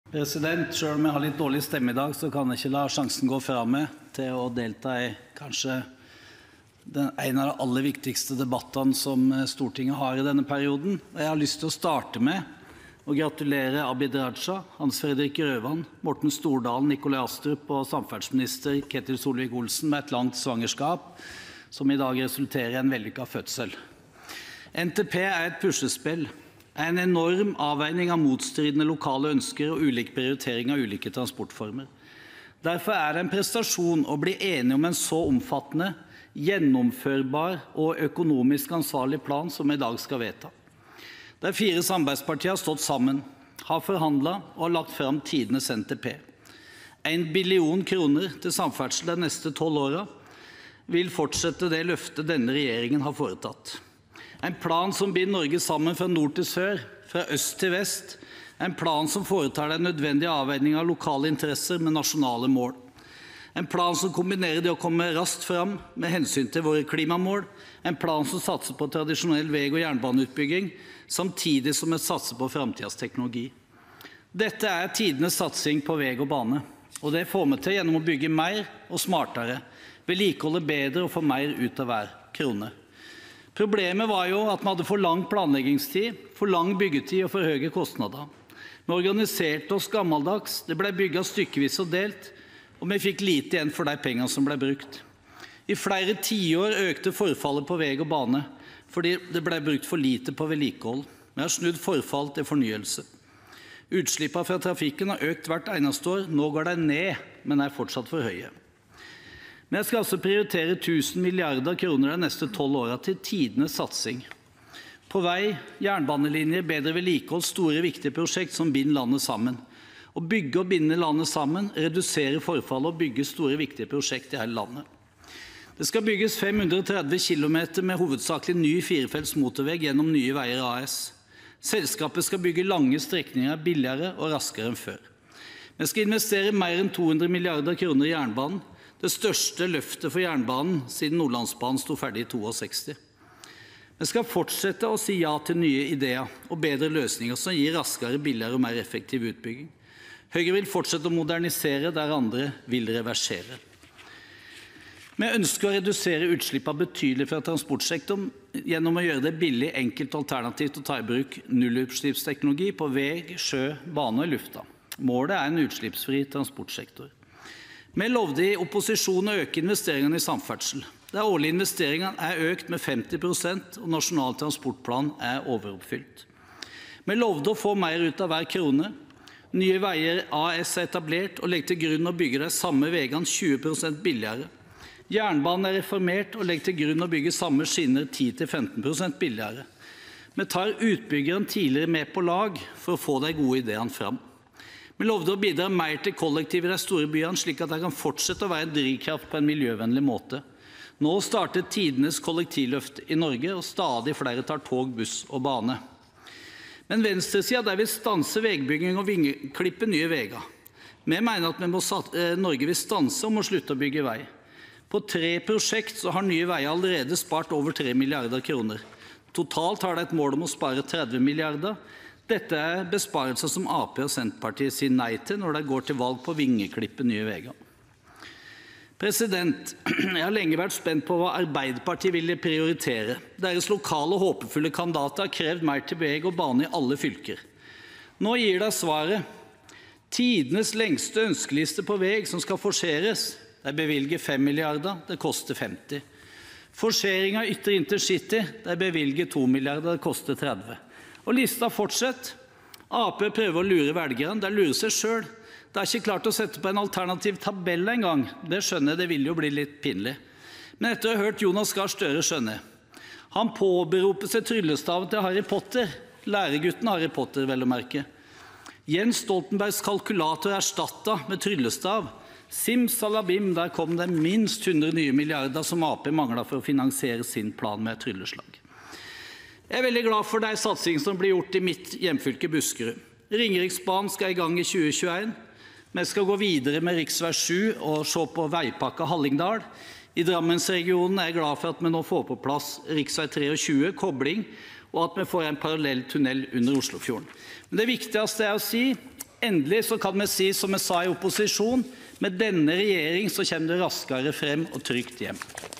19.06.2017 Sak 3 12 Trond Helleland
Om Nasjonal transportplan 2018–2029 (Innst. 460 S (2016–2017), jf. Meld. St. 33 (2016–2017))